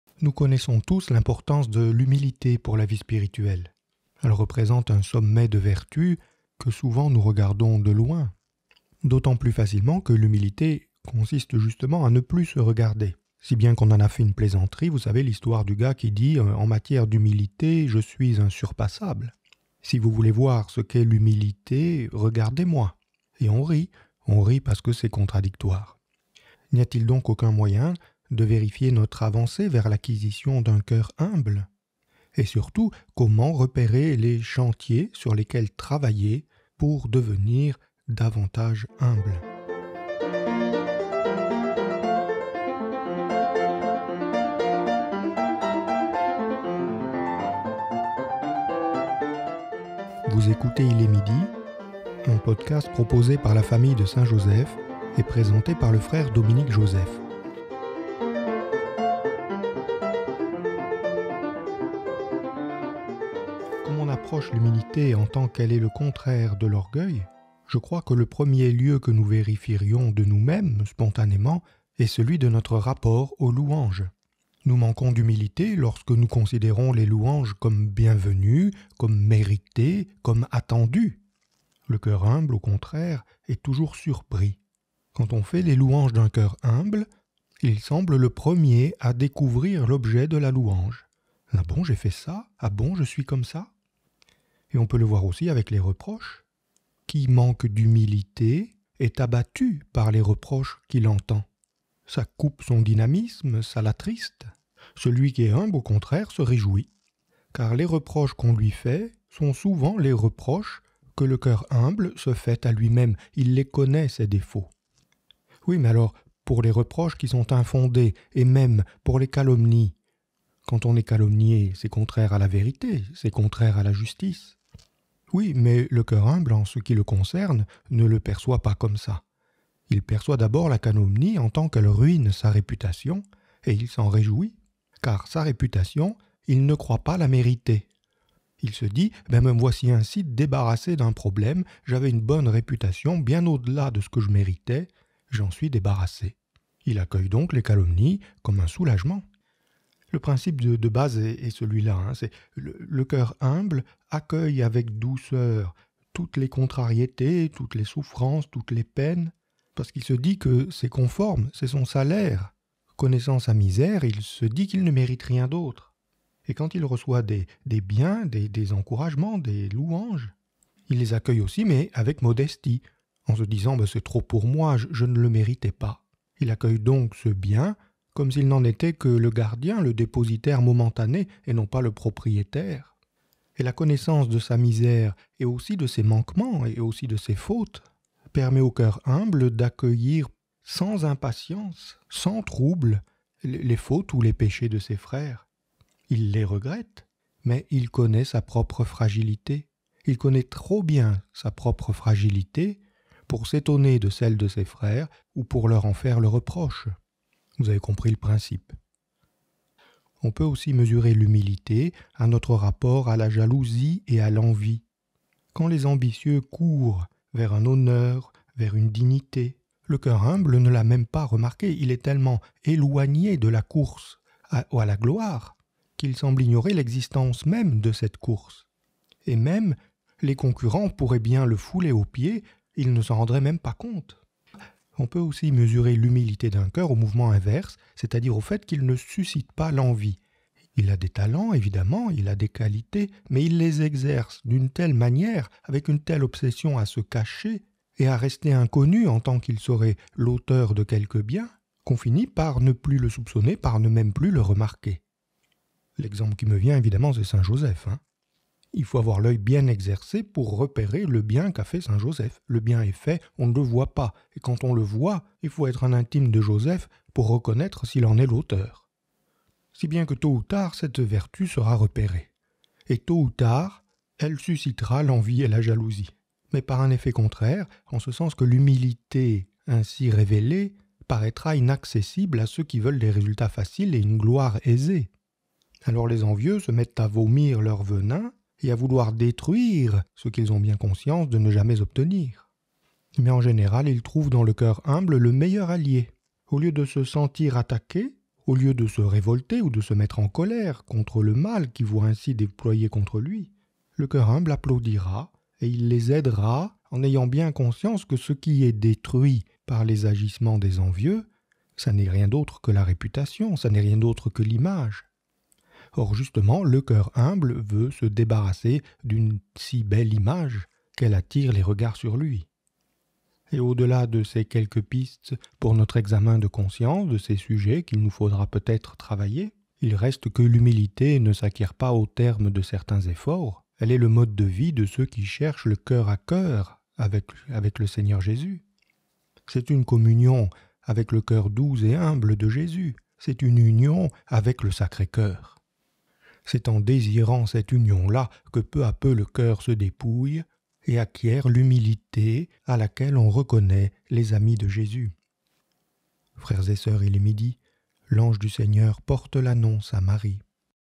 Reconnaître un cœur humble
Saison 2 Épisode 091 - Reconnaître un cœur humble
« Revêtez-vous tous d’humilité dans vos rapports mutuels » (1P 5,5)

L’humilité ne vient pas sans travail : à ses réactions aux louanges et aux reproches, à la jalousie et à l’envie, on reconnaît un cœur humble. Mais c’est le désir de communion avec le Sacré-Cœur qui rend le cœur humble comme le sien.